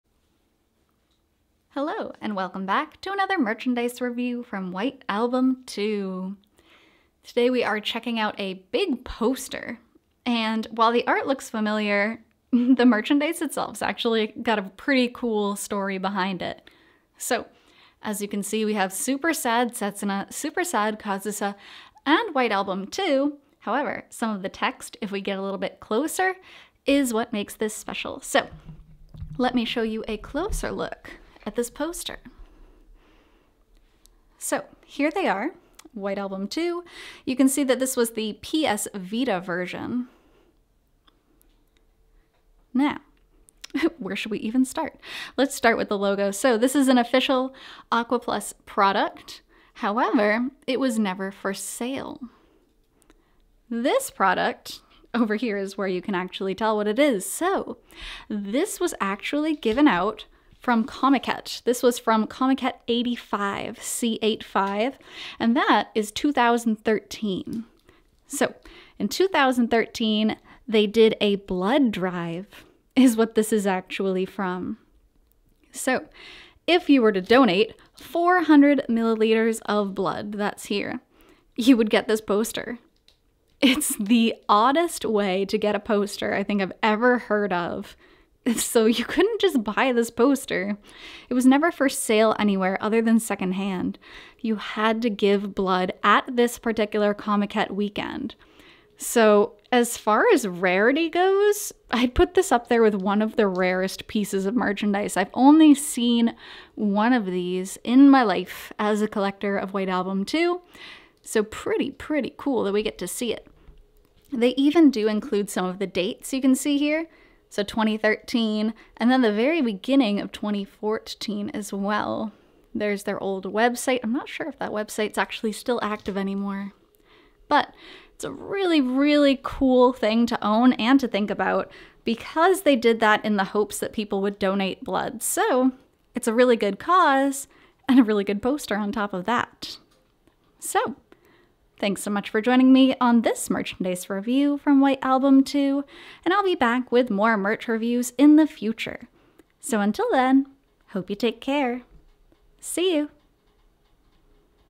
White Album 2 Blood Donation Bonus Poster - C85
A poster with a rather unique story behind it- here's a poster of Kazusa and Setsuna that was given out at Comiket 85 if you donated blood over the weekend!